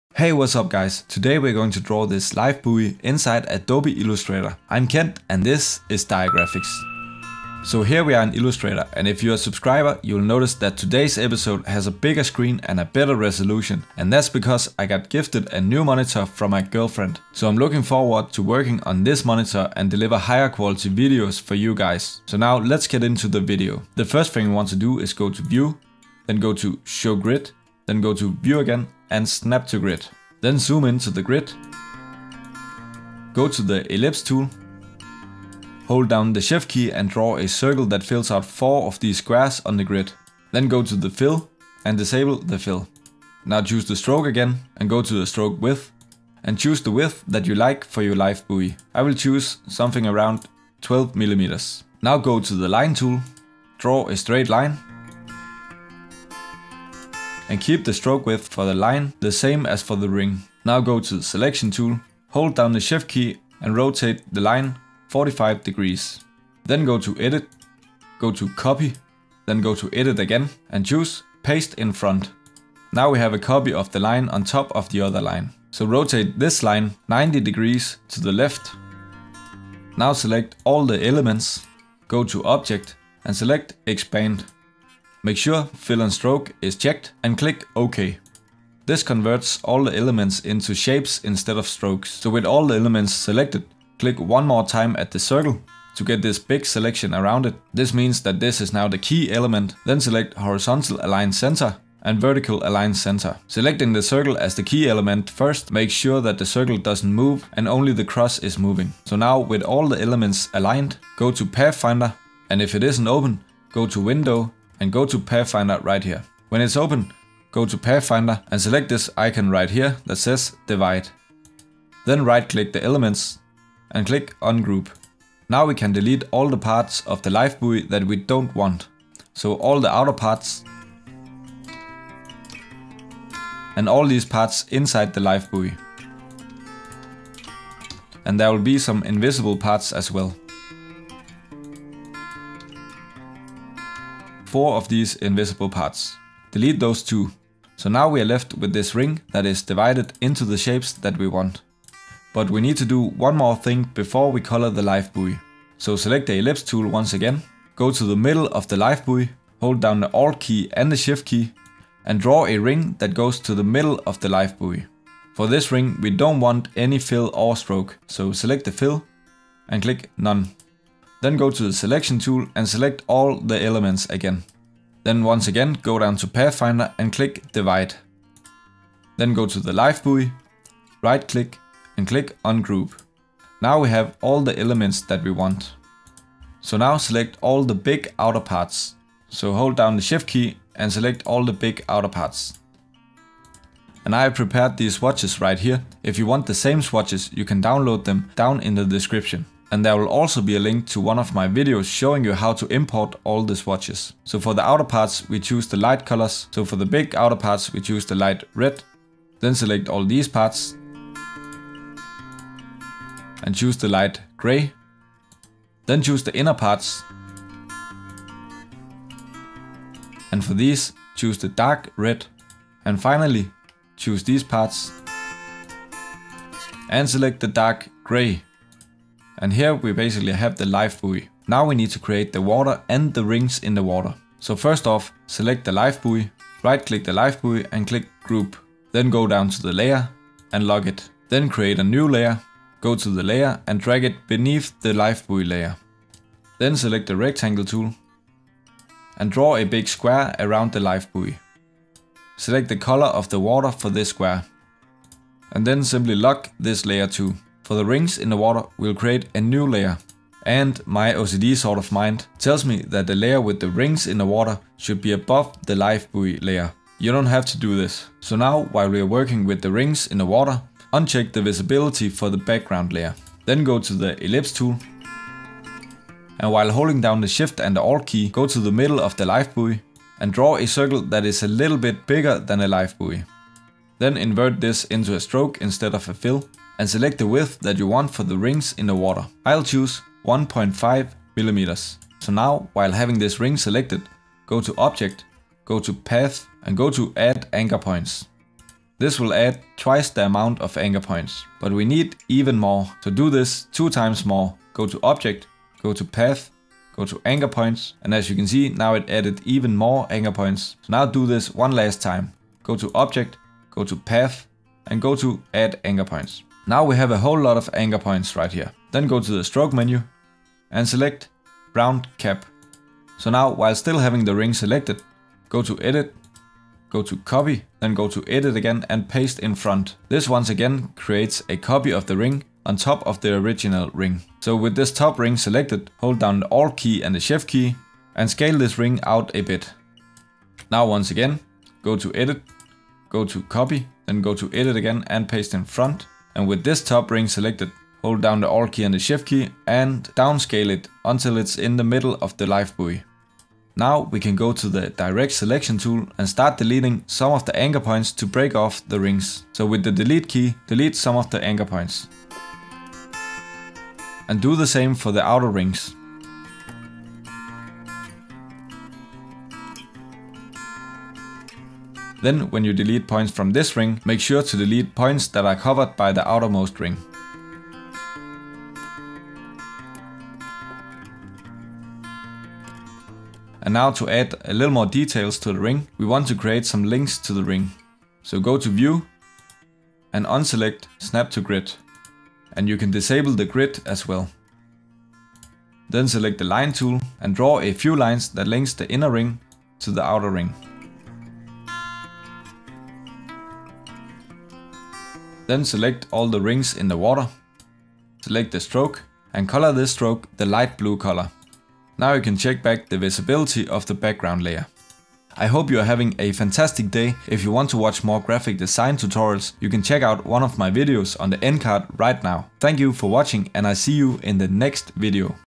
How to Draw a Lifebuoy in Adobe Illustrator: Step-by-Step Tutorial
Learn how to draw a lifebuoy in Adobe Illustrator with this step-by-step tutorial! Whether you're a beginner or experienced designer, you'll find this video easy to follow along with. Join me as i  demonstrate the process of creating a lifebuoy in Adobe Illustrator. Discover how to enable the grid, draw the shapes, align elements, use the Pathfinder tool, add colors, and create water and rings. Improve your graphic design skills and create high-quality illustrations. Watch the video now for a comprehensive guide on drawing a lifebuoy in Adobe Illustrator.

SUMMARY:
-  Enable grid and snap to grid in Adobe Illustrator to facilitate drawing.
-  Use the ellipse tool to draw a circle for the lifebuoy, adjust stroke width as desired.
-  Draw a straight line, rotate it 45 degrees, duplicate it, and rotate the duplicate 90 degrees to create a cross shape.
-  Select all elements, expand them into shapes, and align them using horizontal and vertical alignment tools.
-  Use the Pathfinder tool to divide the elements and delete unwanted parts of the lifebuoy.
-  Draw a ring inside the lifebuoy using the ellipse tool without fill or stroke.
-  Add color to different parts of the lifebuoy using light and dark shades.
-  Create the water and rings in a separate layer using rectangles and circles, adjusting stroke width and anchor points.
-  Delete anchor points to break off the rings and add linking lines between them.
-  Color the rings in the water with a light blue stroke.
-  Lock layers to prevent accidental modifications.
-  Consider using additional resources like swatches and importing techniques for more design options.

📒 Notes 📒

CTRL = Command
ALT = Option